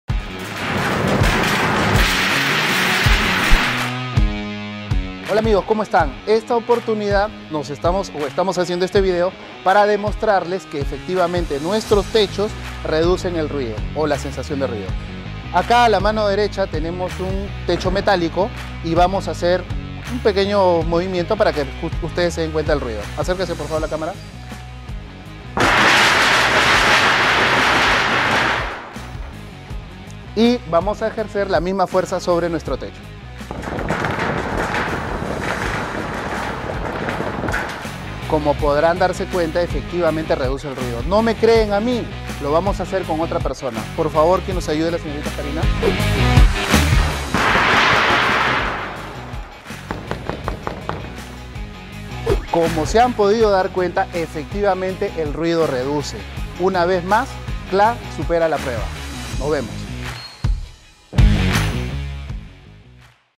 Klar a prueba, reduce el ruido 🔉
Nuestras coberturas Termoacústicas Multicapa Klar posee un aislamiento acústico del 15% superior a las coberturas metálicas tradicionales. 😉

¿Increíble cierto? Una vez más Klar superó la prueba 👏